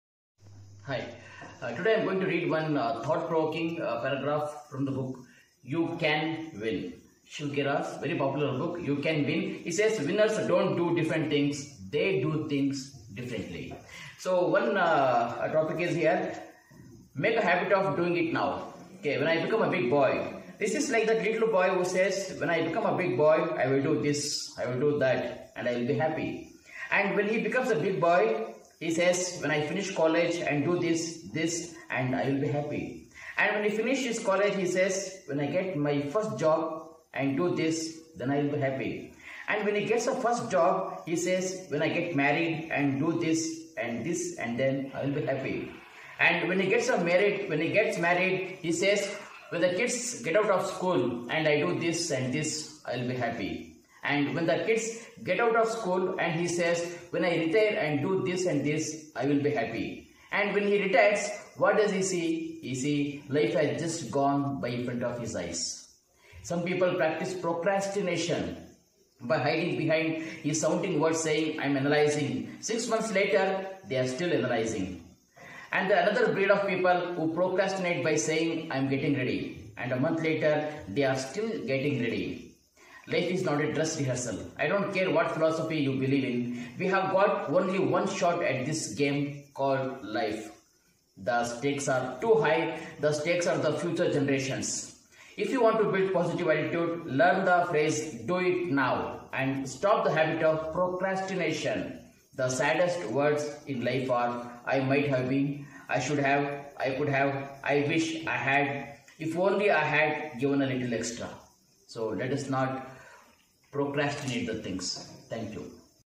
Story Time: :"You Can Win" by Shiv Khera.
Story Time: :"You Can Win"  by Shiv Khera. “Winners don’t do different things, they do things differently” –This  book is all about doing things differently. There are lots of ideas in this book that you can apply to your daily life to ensure your success. More importantly this book will get you excited about life. I'm going to read about Procrastination  : Sridhar Yadravi.